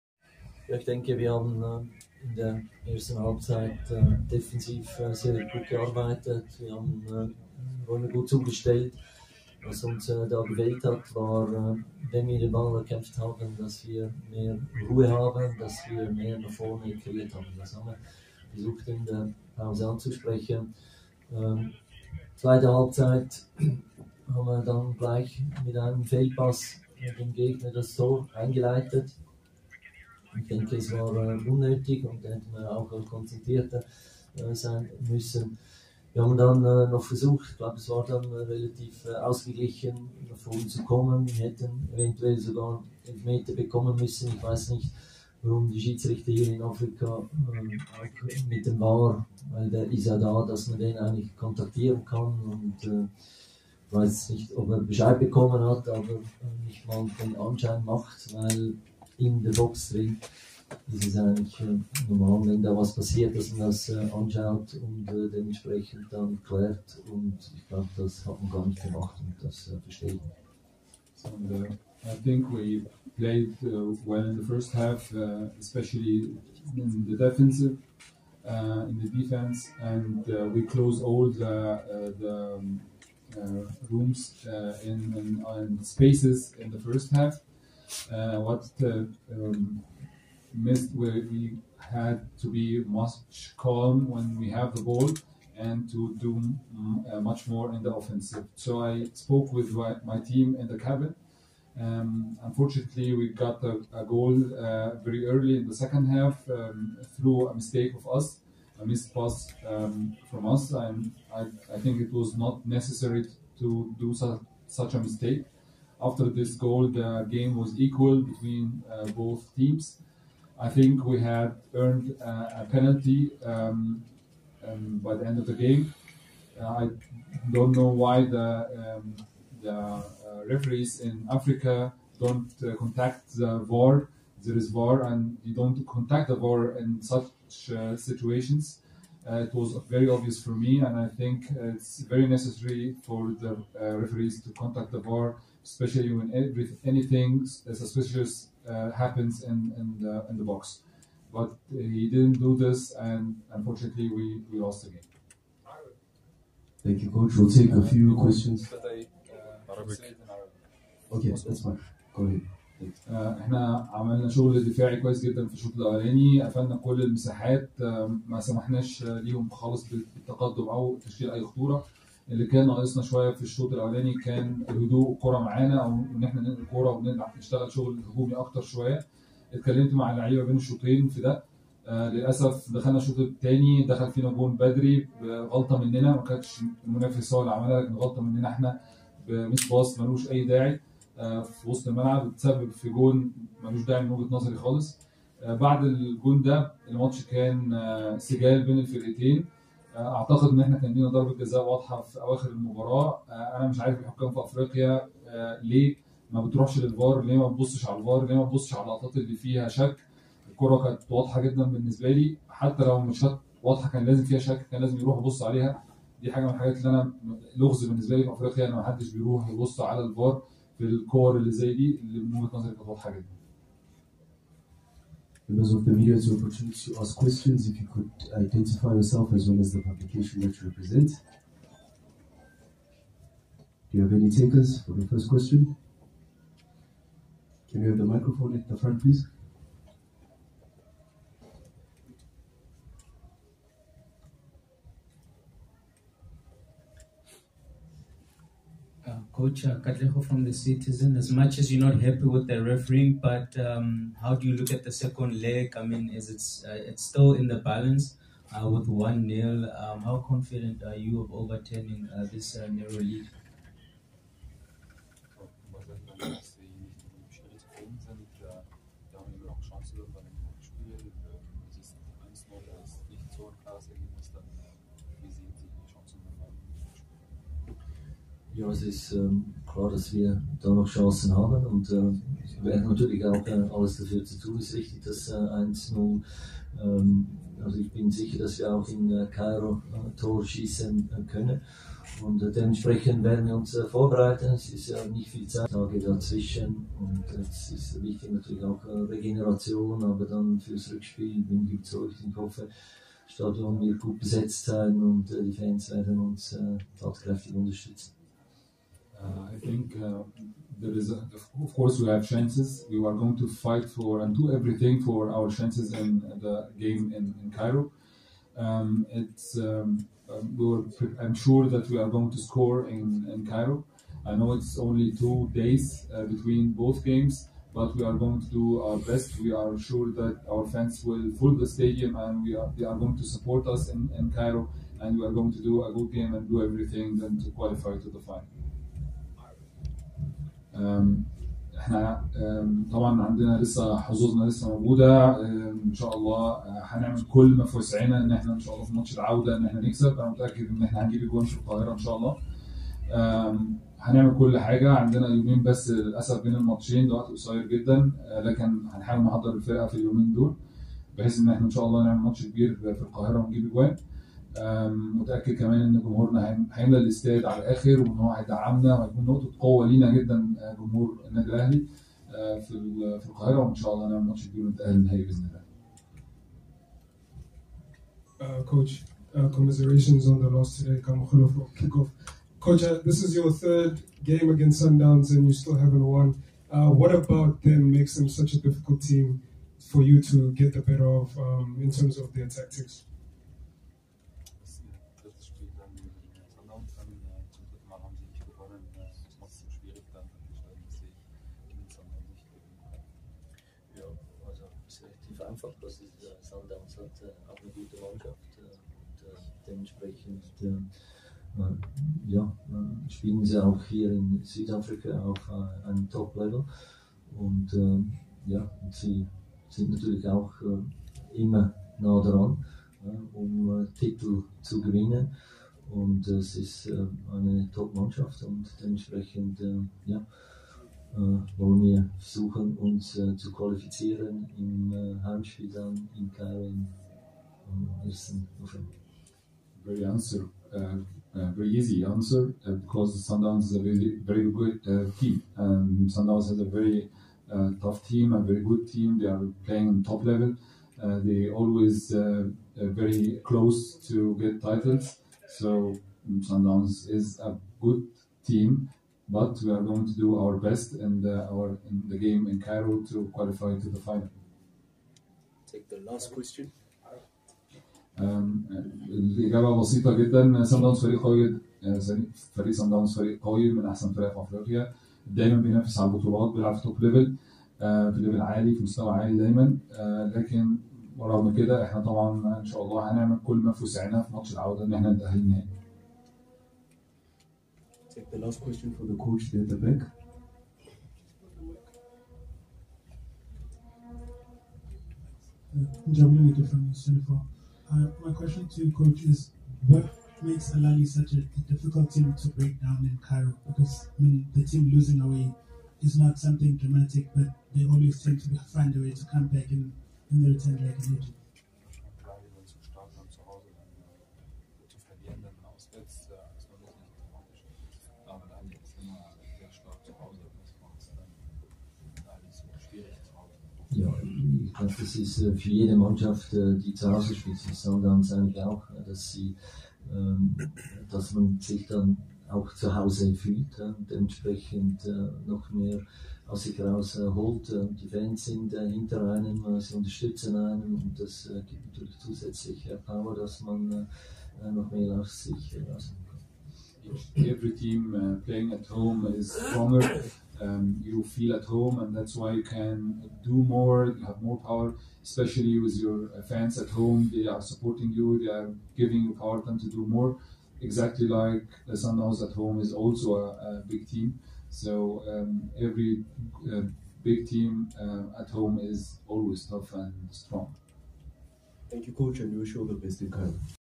Al Ahly coach Marcel Koller reflects on their 1-0 AFL semifinal, 1st leg loss to Mamelodi Sundowns.
Al Ahly coach Marcel Koller reflects on their 1-0 African Football League (AFL) semifinal, 1st leg loss to Mamelodi Sundowns at Loftus.